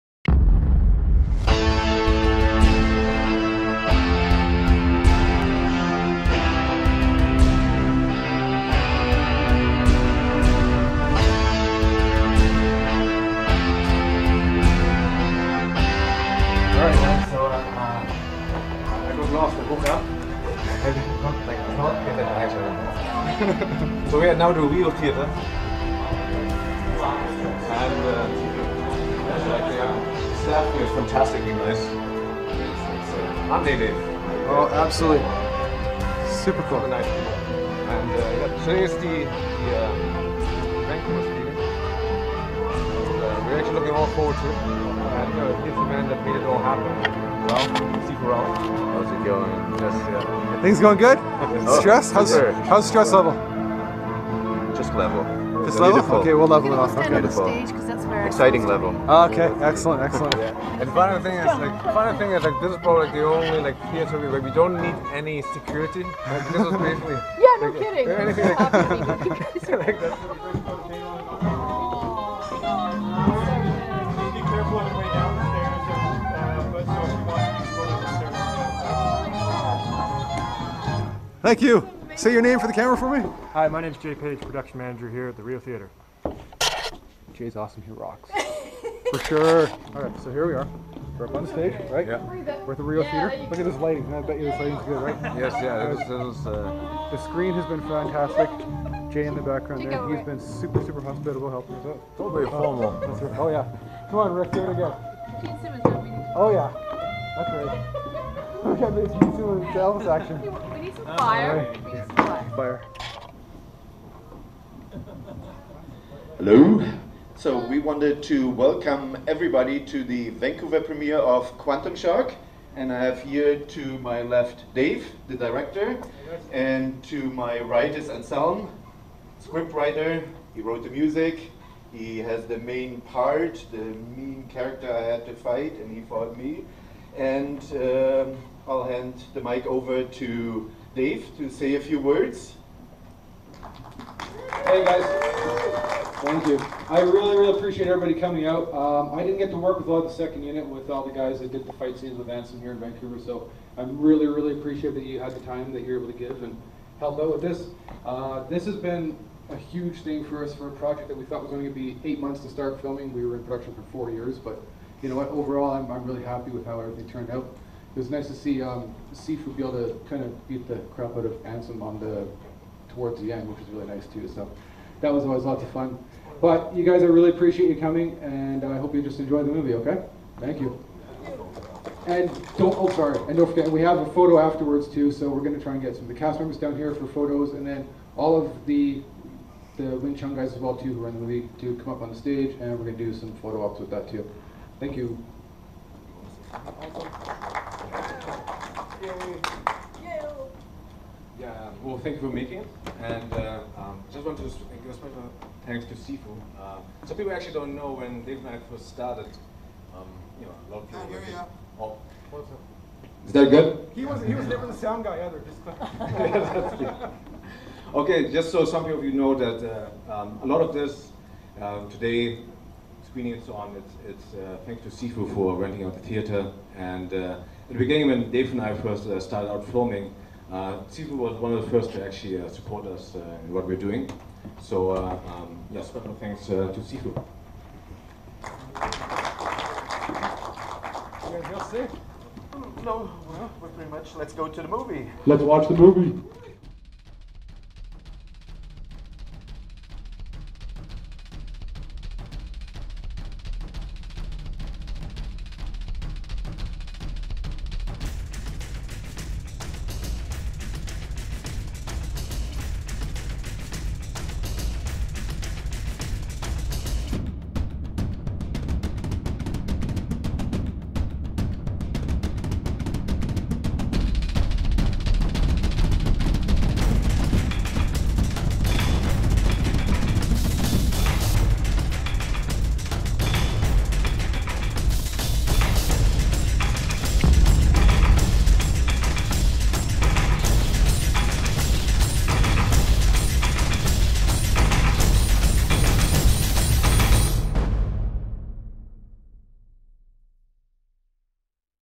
Quantum Shock - The Movie / Premiere at the RIO Theatre
A few shots Dave Campbell, the director, captured on his GoPro at the screening of Quantum Shock - The Movie in Vancouver BC on November 23rd 2014.
The Big-Screen Premiere for the sci-fi action kung-fu movie was held at the RIO Theatre.